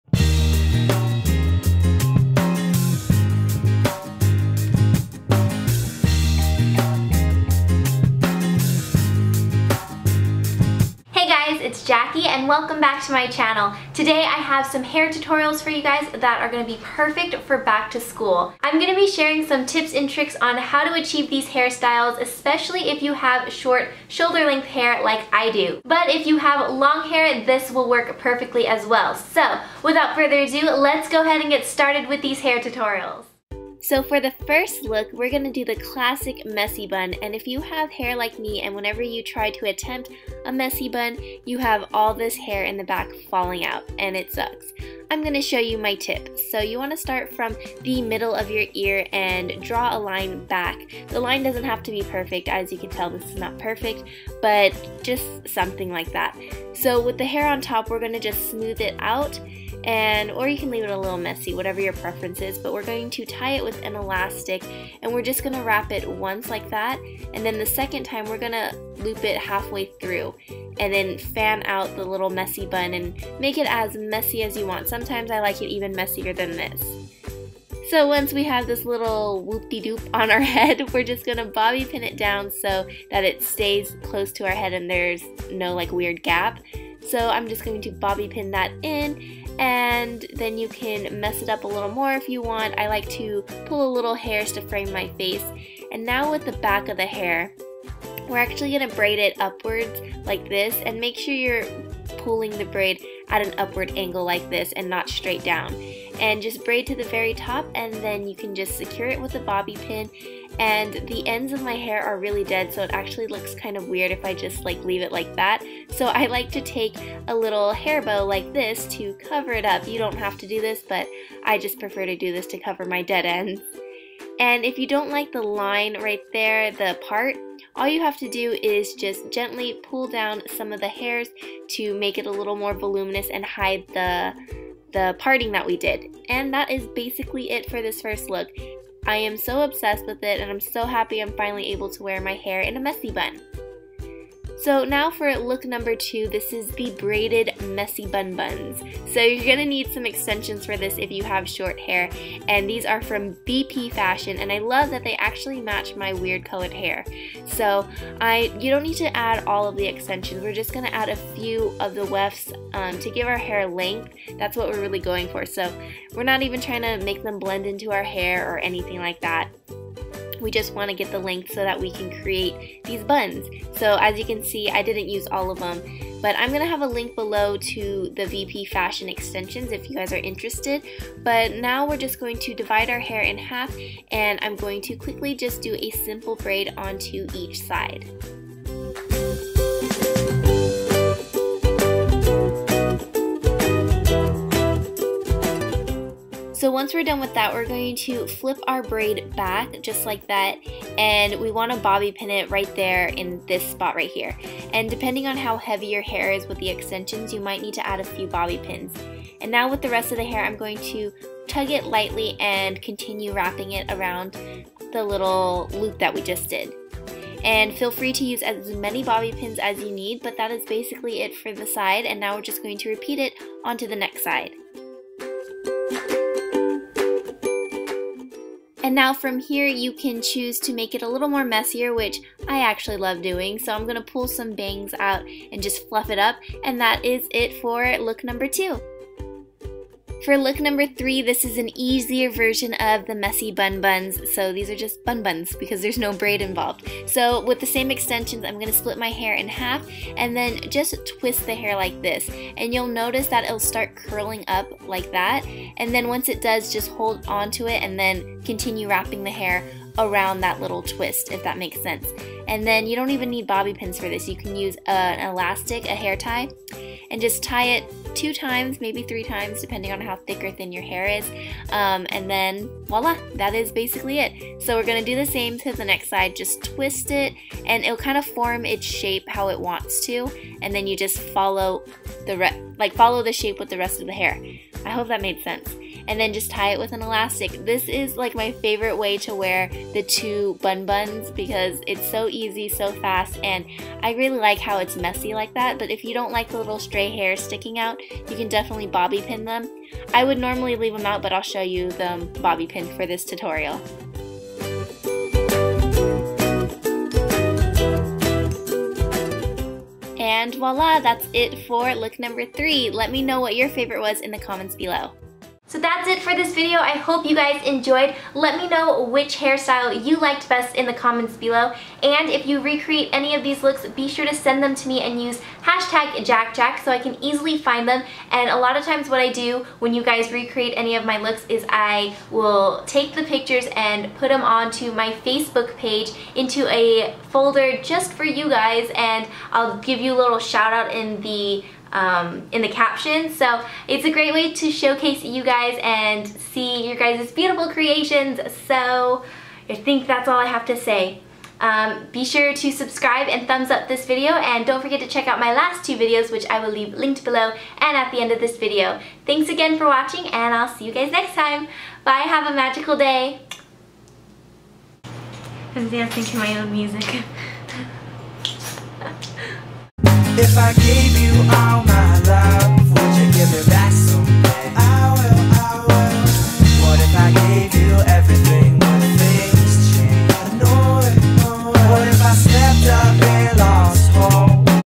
Messy Buns For Short Hair ❤︎
✿ Read on for more information ✿

Hey hey hey, welcome back my lovely little flowers! I'm back with another hair tutorial for you and today I'm going to show you 3 ways to wear messy buns if you have short or medium length hair. Of course you can wear these styles if you have long hair too, but for those who have short hair, I have a few tips and tricks for you to help you achieve these looks! They're super fast, easy and perfect for back to school! Try out these looks and send me a picture using JaaackJack and I'll add you to my Facebook page :)

ps- if you're reading this leave a comment saying "Bun bunz make me happy" hahaha ❤︎

xo Jackie

MAILING ADDRESS
Jackie Perdue
9450 Mira Mesa Blvd. Suite C #510